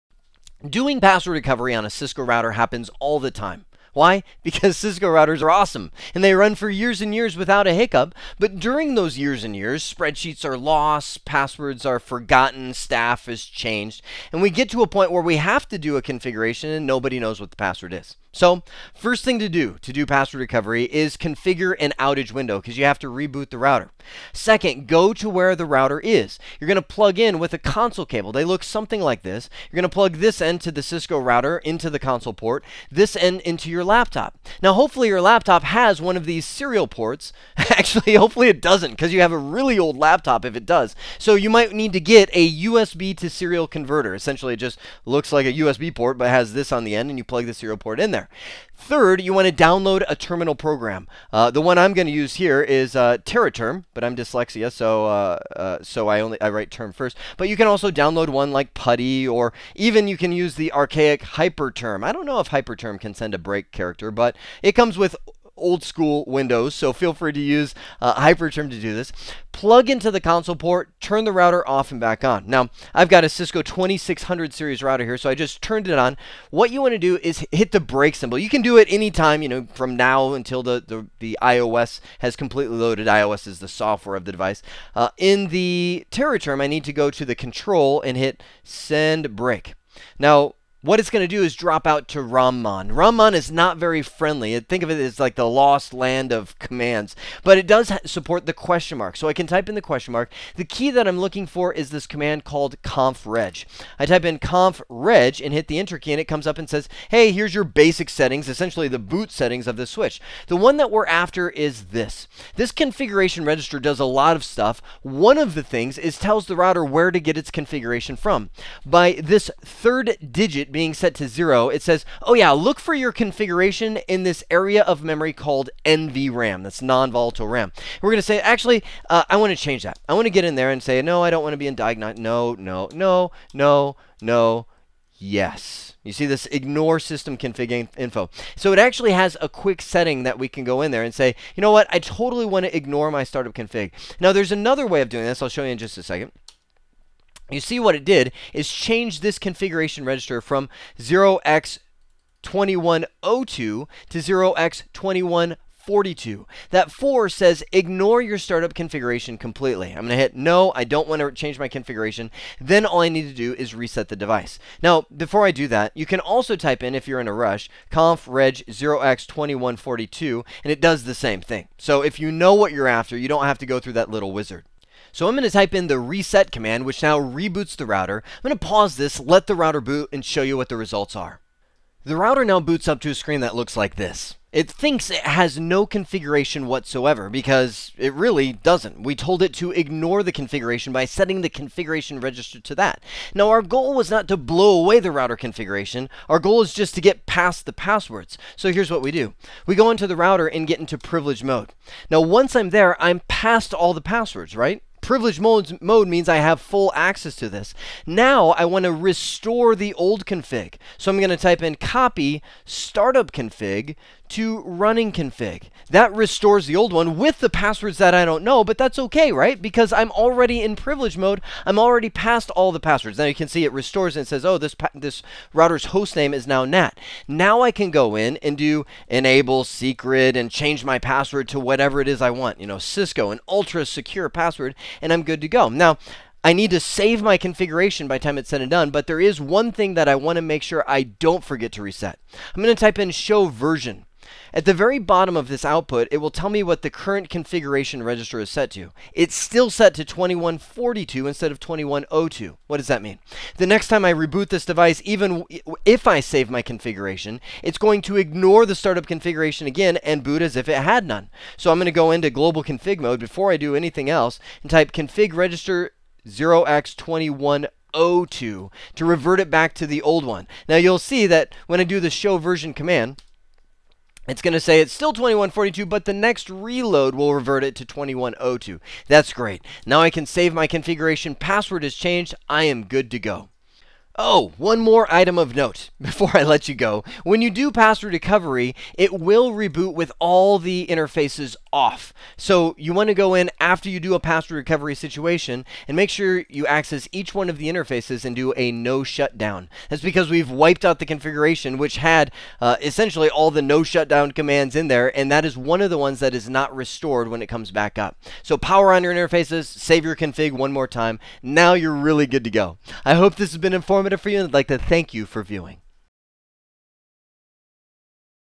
MicroNugget: How to Recover a Password on a Cisco Router
In this video, Jeremy Cioara covers how to recover the password on a Cisco router. Any system administrator who works with Cisco equipment should be familiar with this process: their equipment is so reliable that it will last for years, outlasting many sysadmins and their passwords, which often end up lost.

Cisco routers are some of the most reliable equipment you’ll encounter, but there’s a bit of a downside to this. The equipment usually lasts through multiple sysadmin switchovers and, because the router is often running for long periods without needing anything, passwords invariably get lost or forgotten. In this step by step walkthrough, you’ll learn a quick, straightforward process for recovering a password in this situation. You’ll begin by rebooting the router, then plug in a console cable between the router and the laptop. At this point, there are a series of commands you need to key in to retrieve the information you’re looking for.